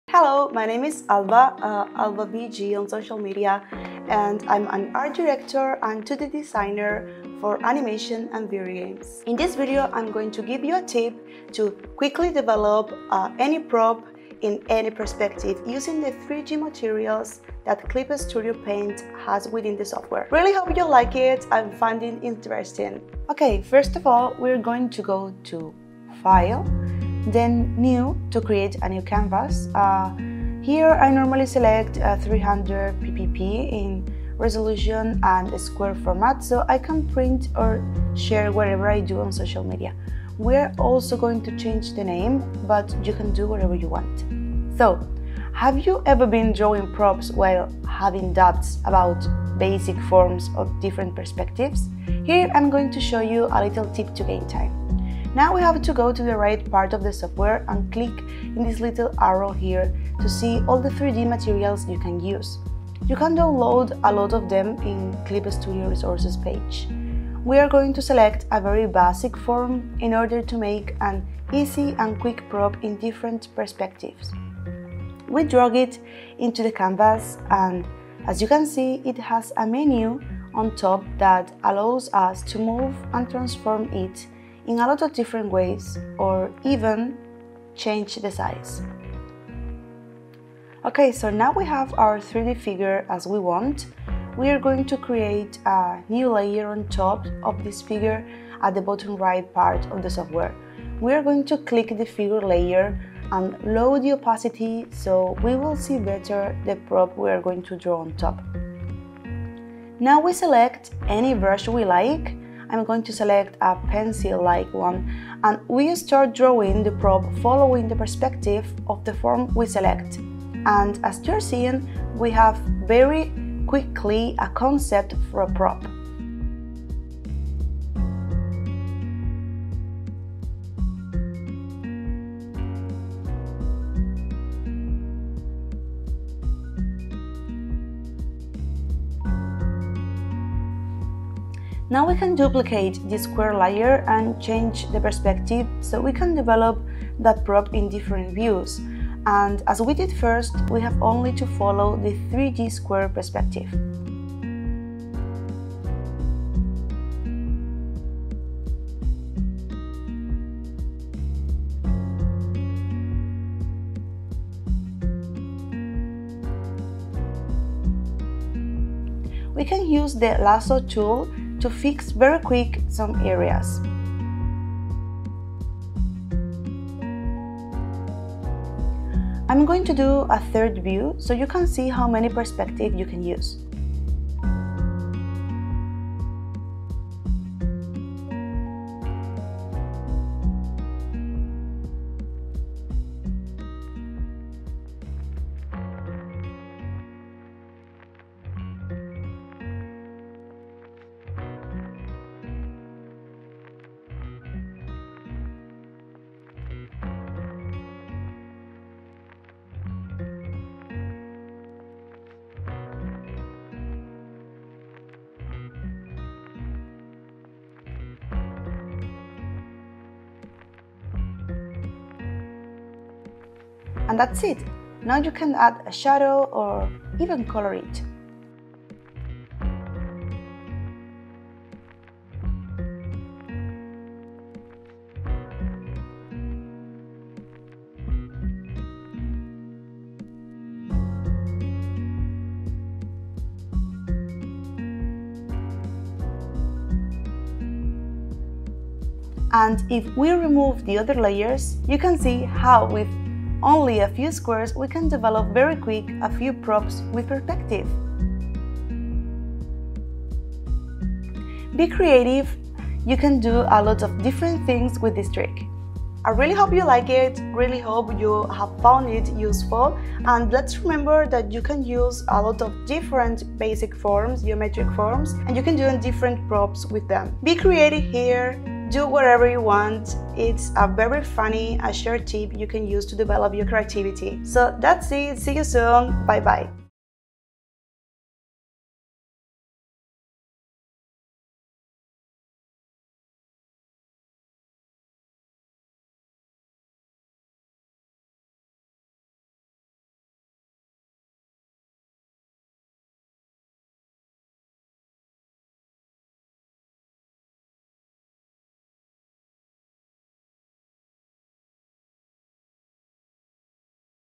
Make quick concept art with this exercise! | AlbaBG
Download the free assets here!

Artist and animator AlbaBG introduces a technique to make concept art and turnarounds using basic 3D models! Learn how you can make your first turnaround using a cube, and take your first step as a
concept artist!